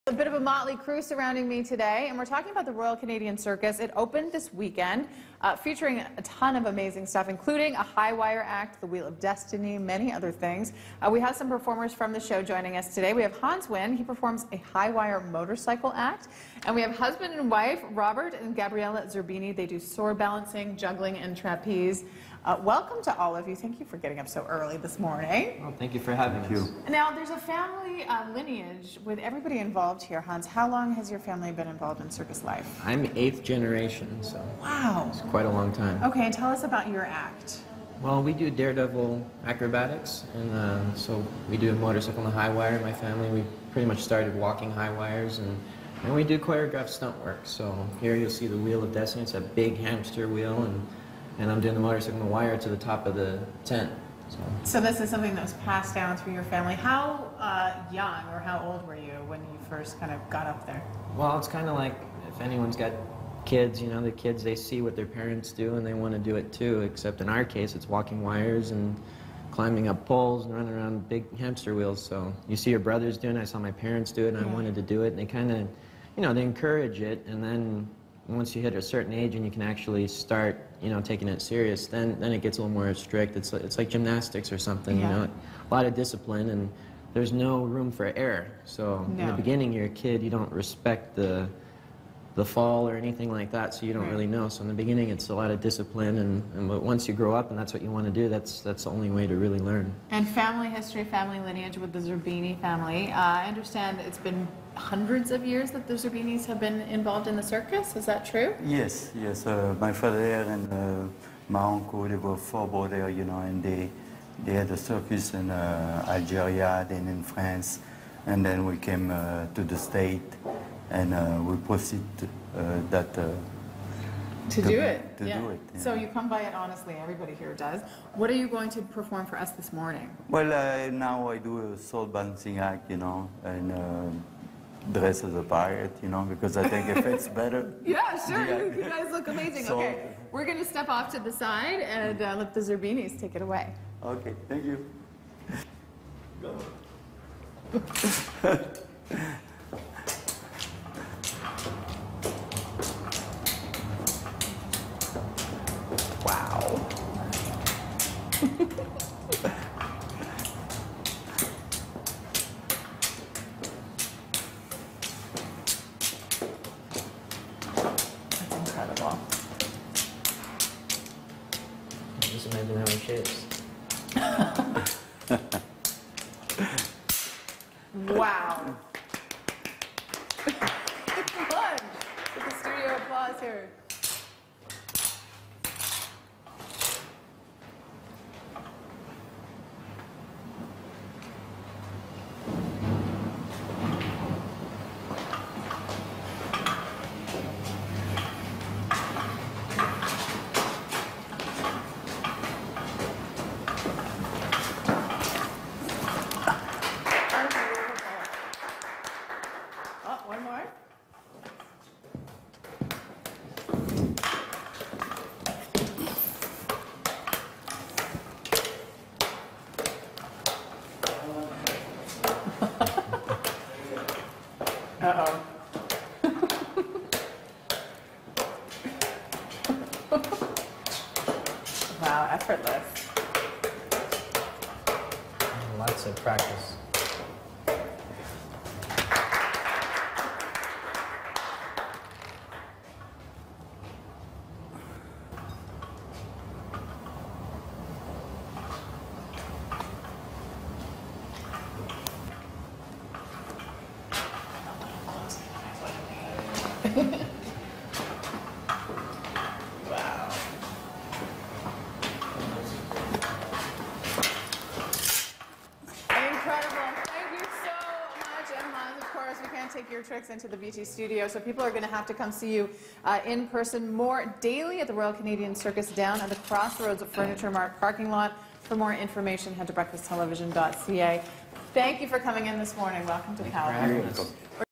Royal Canadian Circus Preview - May 12th
Juggling knives, balancing swords ... The Royal Canadian Circus visits the BT studio for a sneak peek.